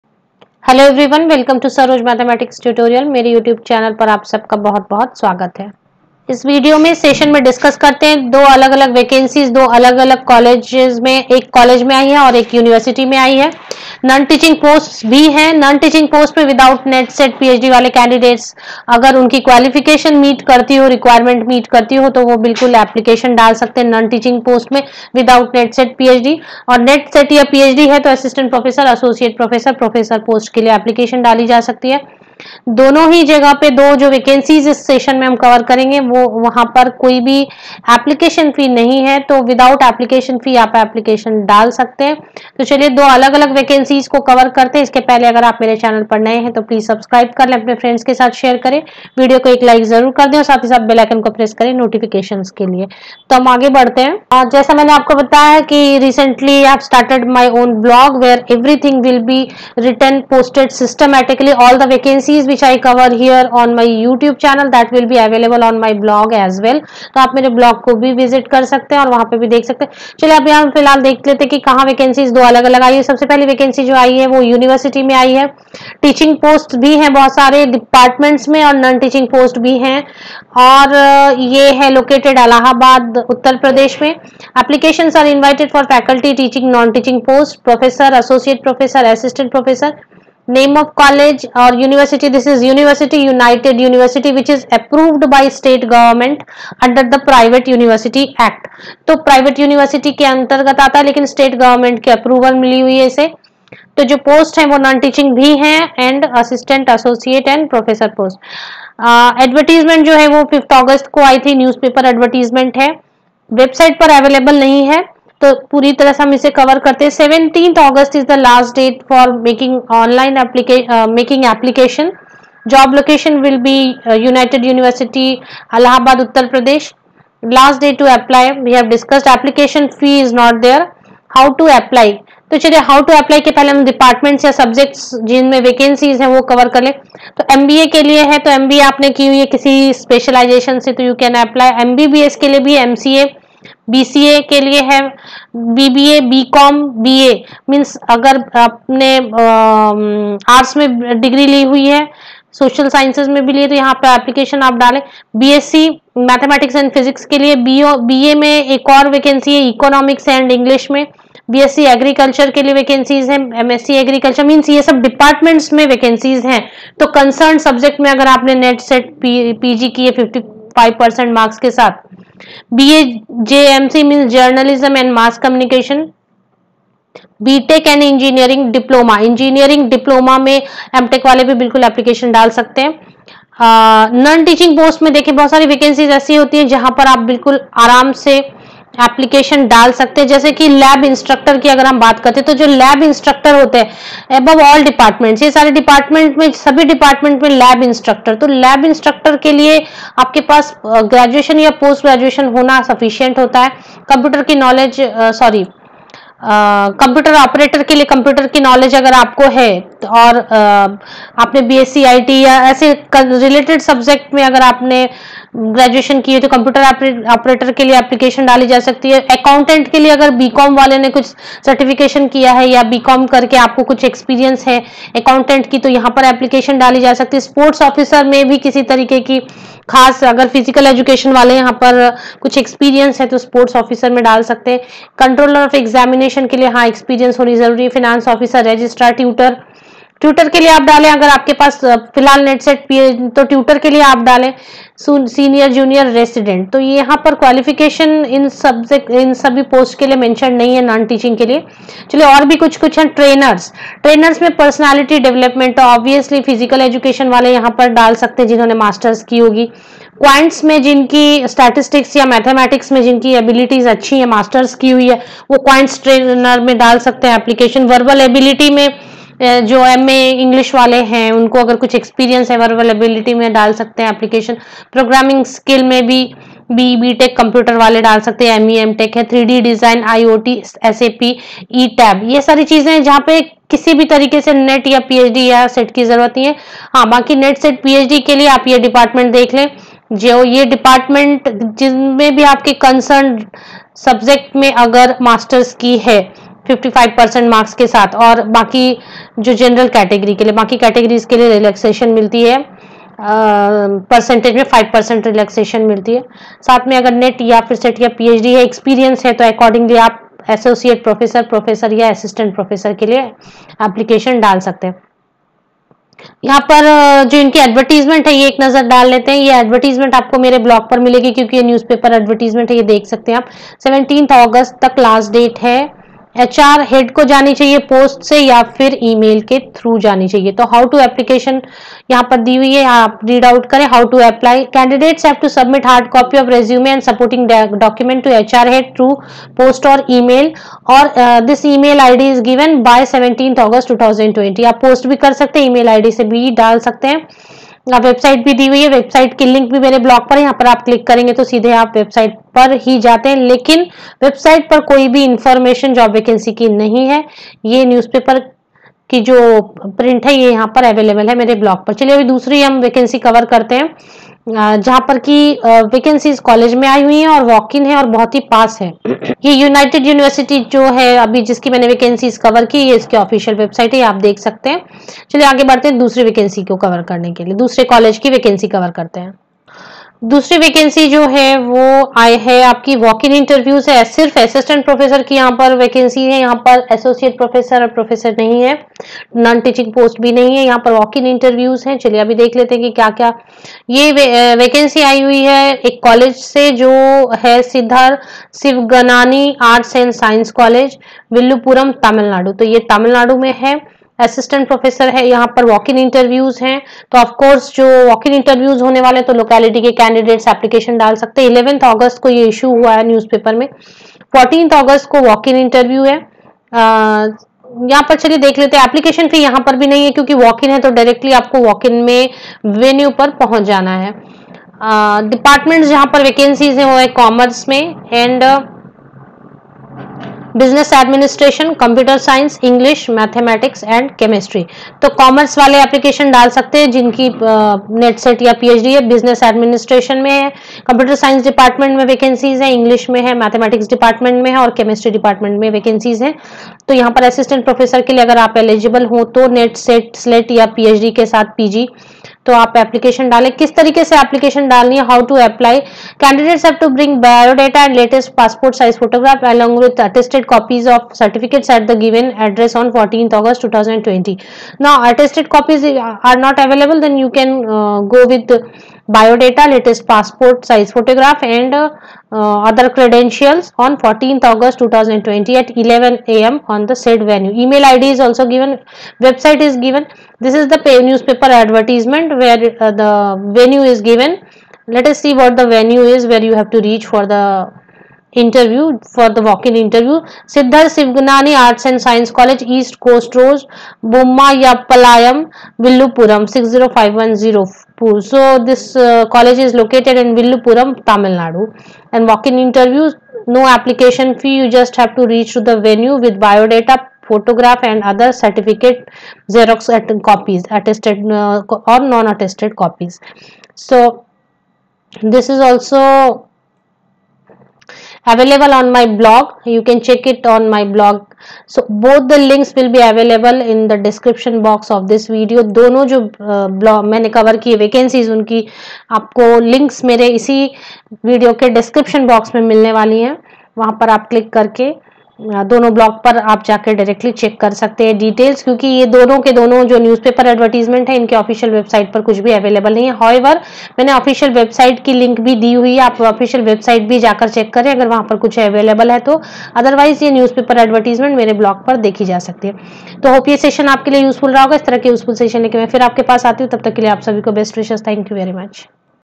Vacancies WITH & WITHOUT NET/SET/PHD / Vacancies in TWO Colleges / NO Application Fee/ WALK- IN
1. Below is Blog link to see the details for Siddhar Sivagnaani Arts and Science College, Villupuram, Tamilnadu

2.. Below is Blog link to see the details for United University, Allahabad